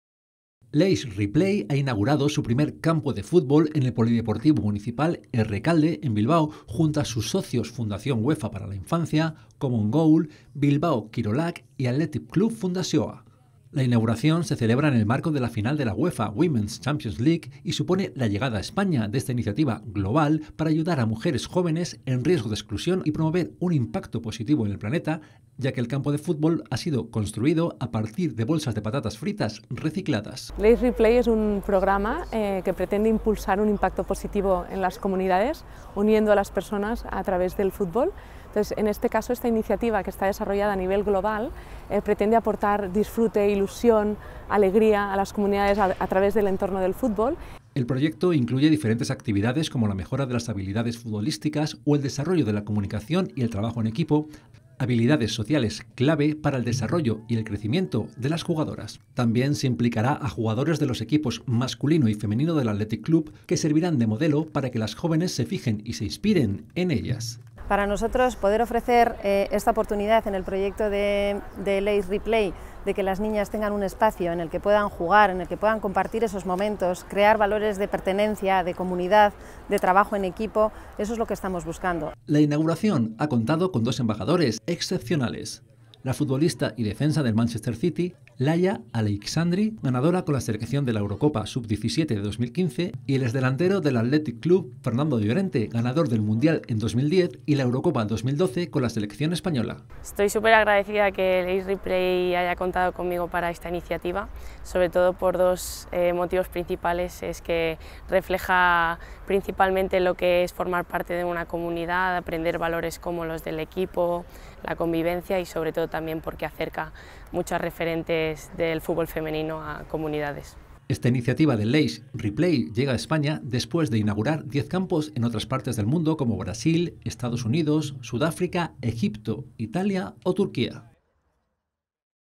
Lay’s RePlay apoya a mujeres jóvenes en riesgo de exclusión a través del fútbol en Bilbao
Lay's RePlay ha inaugurado su primer campo de fútbol en el polideportivo municipal Errekalde, en Bilbao, junto a sus socios Fundación UEFA para la Infancia, Common Goal, Bilbao Kirolak y Athletic Club Fundazioa.

La inauguración se celebra en el marco de la final de la UEFA Women's Champions League, y supone la llegada a España de esta iniciativa global para ayudar a mujeres jóvenes en riesgo de exclusión y promover un impacto positivo en el planeta, ya que el campo de fútbol ha sido construido a partir de bolsas de patatas fritas recicladas.

El proyecto incluye diferentes actividades como la mejora de las habilidades futbolísticas o el desarrollo de la comunicación y el trabajo en equipo, habilidades sociales clave para el desarrollo y el crecimiento de las jugadoras.

También se implicará a jugadores de los equipos masculino y femenino del Athletic Club, que servirán de modelo para que las jóvenes se fijen y se inspiren en ellas.

La inauguración ha contado con dos embajadores excepcionales: la futbolista y defensa del Manchester City Laia Aleixandri, ganadora con la selección de la Eurocopa sub-17 de 2015, y el ex delantero del Athletic Club Fernando Llorente, ganador del Mundial en 2010 y la Eurocopa 2012 con la selección española.

Esta iniciativa de Lay's RePlay llega a España después de inaugurar diez campos en otras partes del mundo, como Brasil, Estados Unidos, Sudáfrica, Egipto, Italia o Turquía.